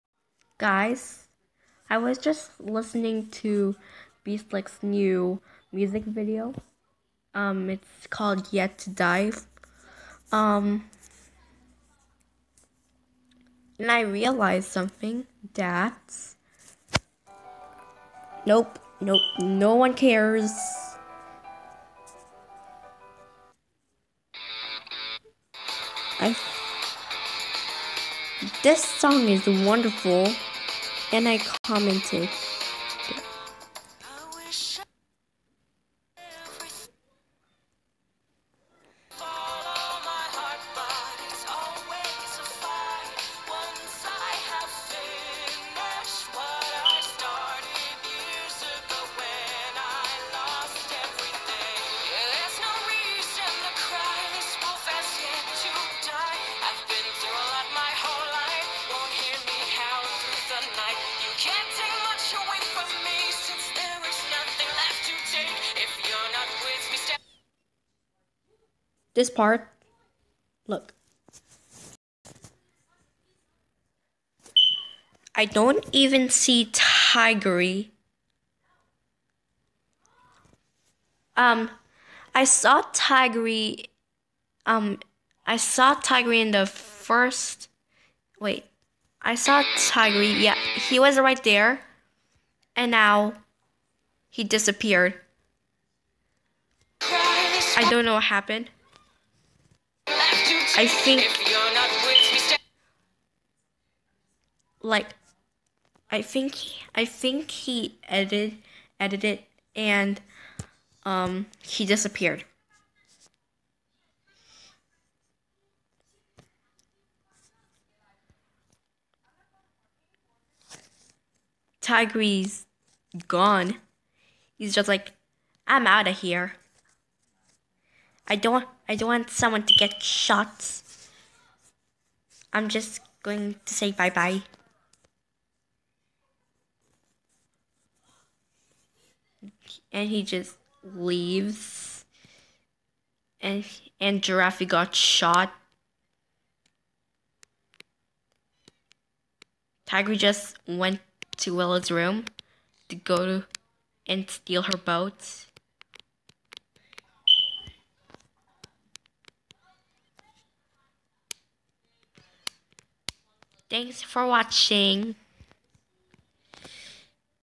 I just realized something on @BSlick's new music video
where in the world did that tiger go? I just realized that.

my roblox username: jcxa9